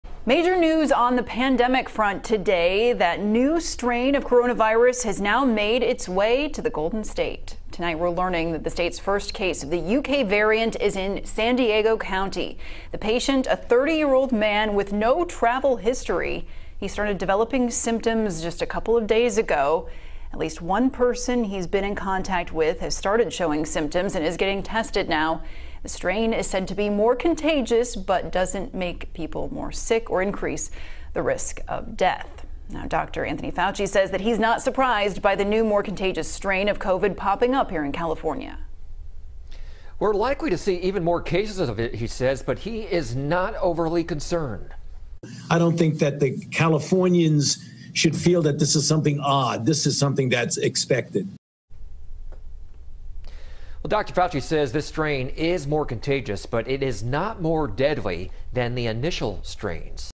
Dr. Fauci Says New Strain Of COVID Is Not More Deadly Than Others
The strain was only recently confirmed in the US this week, with the first case being confirmed in Colorado.